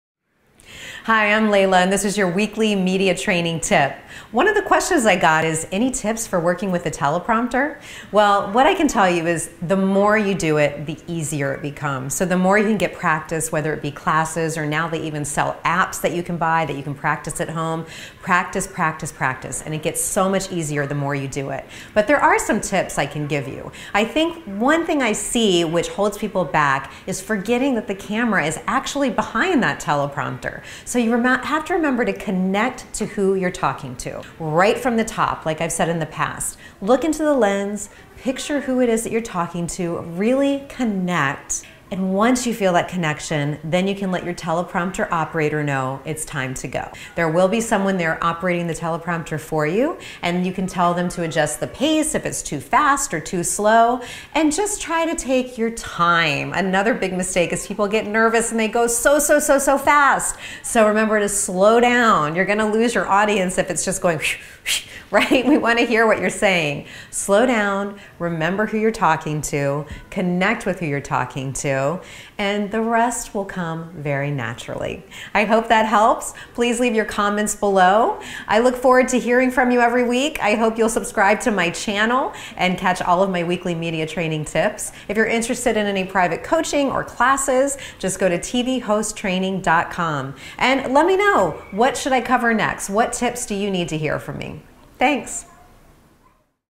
How to be a TV Host- Working with a Teleprompter
This is the best tip for working with a teleprompter on camera.